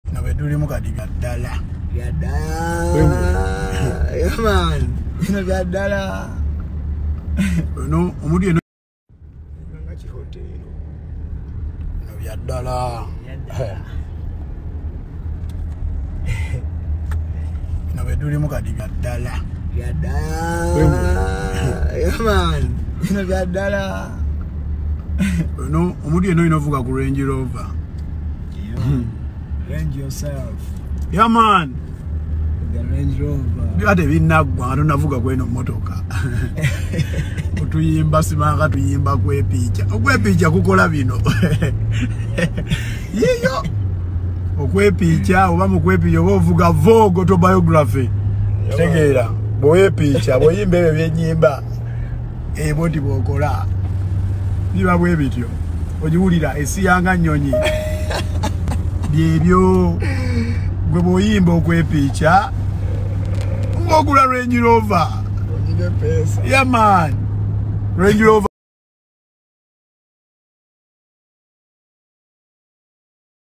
okwepincha singer gravity omutujju aguze range rover Vogue laba esanyu
omuyimbi gravity omutujju aguze emotoka empya esanyu kata limutte